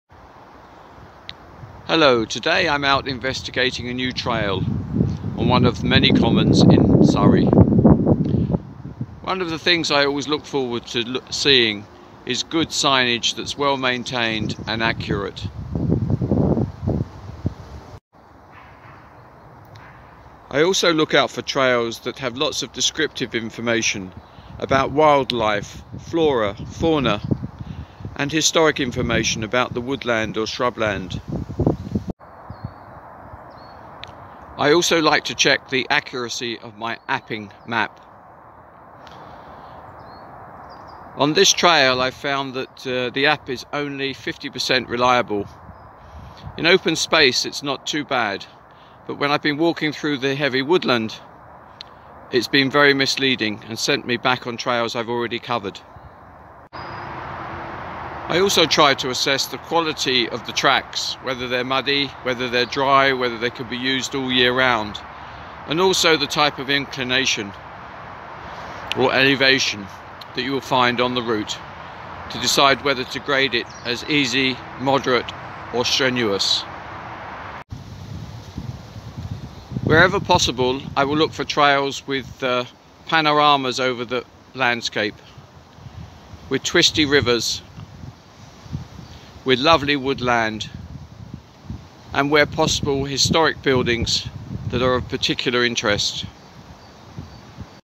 Assessing Countryside Trails
We are often investigating new trails for the Channel and people often ask what we consider before deciding to produce a trail Self-Guide.

One of the first things we look forward to seeing is good signage that is well maintained and accurate. We also look out for trails  that have lots of descriptive plaques about wild-life, flora, fauna and historic information about the woodland or shrubland.

IWe like to check the accuracy of our mapping app. On one trail we found the app only 50% reliable. In open space it wasn t too bad but when walking through heavy woodland it was misleading and sent us back to trails we had already covered.

We also try to assess the quality of the tracks. Whether they are muddy; whether they are dry and can be used all year round and also the inclination or elevation you will find on the route to decide whether to grade it easy, moderate or strenuous.

Where ever possible we will look for trails with good panoramas over the landscape; with twisty rivers, with lovely woodland  and where possible historic buildings that are of particular interest.